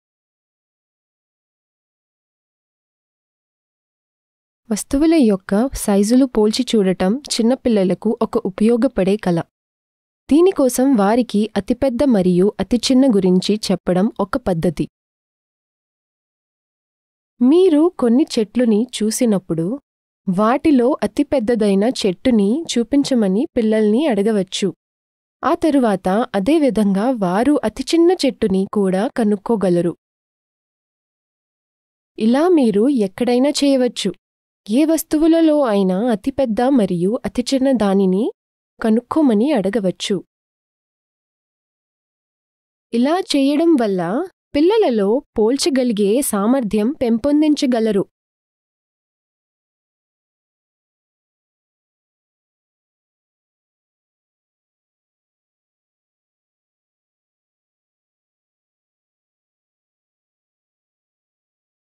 అతిపెద్ద మరియు అతిచిన్నది గా పోల్చడం - Comparing Biggest and Smallest (Telugu)
పరిమాణాలను పోల్చడం చిన్న పిల్లలకు ఒక ఉపయోగకరమైన నైపుణ్యం. దీన్ని చెయ్యటానికి ఒక పద్ధతి, "అతిపెద్ద", "అతిచిన్న",గురించి మాట్లాడటం. మీరు ఇది ఎక్కడైనా చెయ్యవచు. పిల్లల ని, ఏదైనా ఒక రకమైన వస్తువు లో, అతిపెద్దది, అతిచిన్నది కనుక్కోమని అడగవచ్చు. ఇలా చేయడం వల్ల, విషయాలను పోల్చడం లో పిల్లల సామర్ధ్యాన్ని అభివృద్ధి చేయడానికి మీరు సహాయపడుతున్నారు.

జీవితం కోసం నేర్పడం అనే ఈ ప్రపంచ ఉద్యమం లో, మన పిల్లలకు మంచి విద్యను అందిస్తూ  మరియు ఉన్నత భవిష్యత్తు ని ఆశిస్తూ అందరూ తమ జ్ఞానాన్ని పంచుతున్నారు.

Telugu translation by Kavitha Gorantla and Jyothsna Tamtam. Narration by Vineeta Verma.

A useful skill for young children is comparing sizes. One way to do this is by talking about “biggest” and “smallest.” You can do this almost anywhere. Just ask your child to find the biggest and smallest of a certain type of object. By doing this, you are helping them develop their ability to compare things.